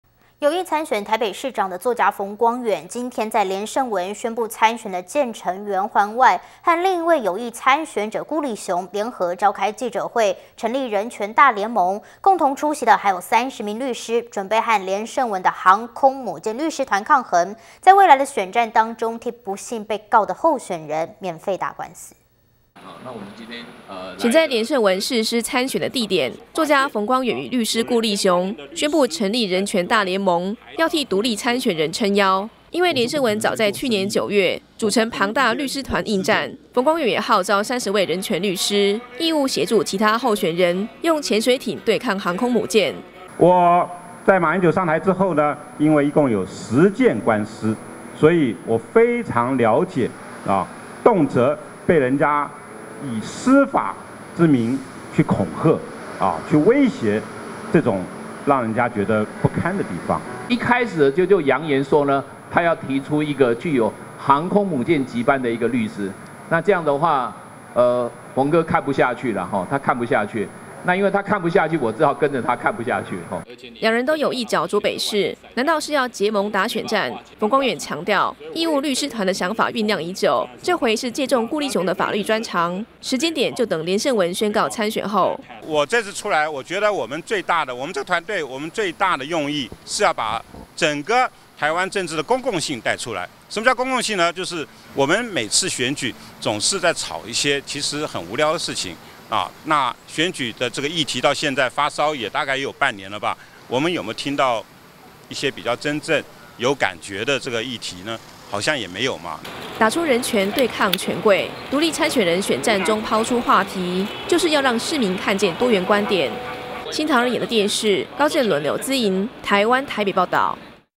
【台灣新聞】馮光遠顧立雄攜手 成立人權大聯盟
【台灣新聞】馮光遠顧立雄攜手 成立人權大聯盟(NTDAPTV)

選在連勝文誓師參選的地點，作家馮光遠與律師顧立雄，宣布成立人權大聯盟，要替獨立參選人撐腰，因為連勝文早在去年9月，組成龐大律師團應戰，馮光遠也號召30位人權律師，義務協助其他候選人，用潛水艇對抗航空母艦。

台北市長參選人 馮光遠：「我在馬英九上台之後，因為一共有10件官司，所以我非常了解，動輒被人家以司法之名去恐嚇，去威脅這種讓人家覺得不堪的地方。」

台北市長參選人 顧立雄：「(連勝文)一開始就揚言說要提出一個具有航空母艦級般的律師，那這樣的話馮哥看不下去，那因為他看不下去，我只好跟著他看不下去。」

倆人都有意角逐北市，難道是要結盟打選戰？馮光遠強調，義務律師團的想法醞釀已久，這回是借重顧立雄的法律專長，時間點就等連勝文宣告參選後。

台北市長參選人 馮光遠：「我這次出來我覺得我們這團隊最大的用意，是要把整個台灣政治的公共性帶出來，什麼叫公共性？就是我們每次選舉，總是在吵一些其實很無聊的事情，那選舉這個議題到現在發燒，大概也有半年了吧，我們有沒有聽到一些比較真正有感覺的這個議題，好像也沒有。」

打出人權對抗權貴，獨立參選人選戰中拋出話題，就是要讓市民看見多元觀點。